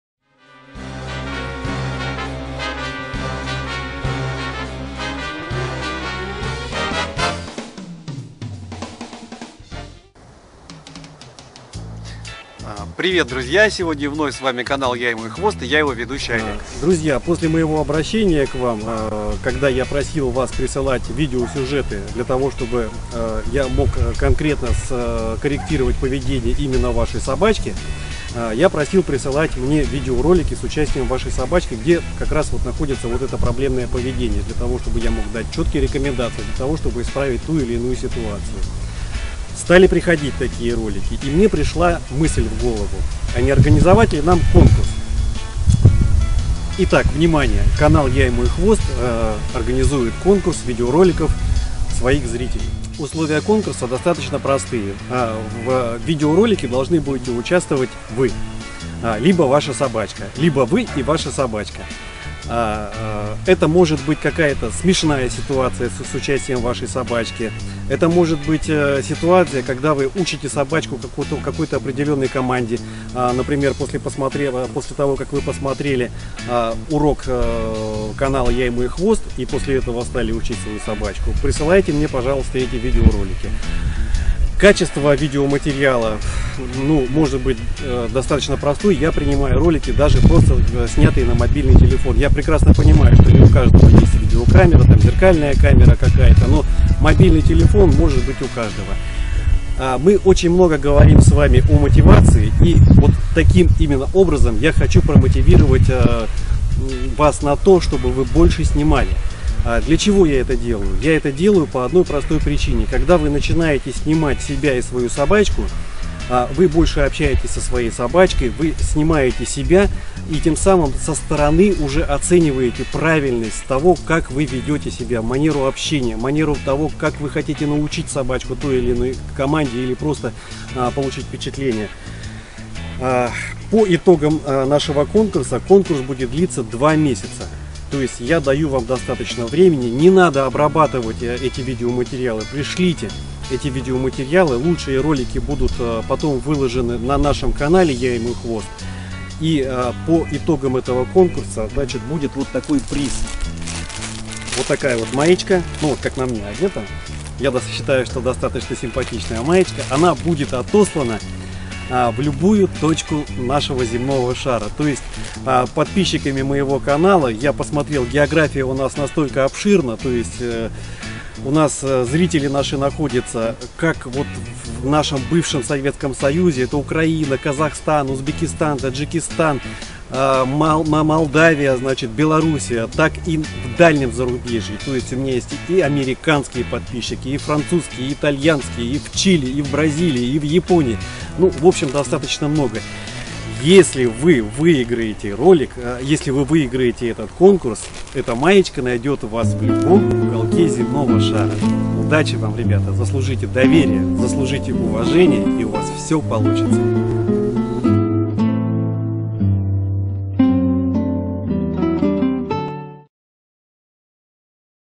ВНИМАНИЕ ! конкурс ВИДЕОРОЛИКОВ!
условия конкурса очень простые! на видео должна быть именно Ваша собачка и ролик должен быть снят Вами, а не скачен с интернета!   Удачи всем! Поддержка канала от 1 рубля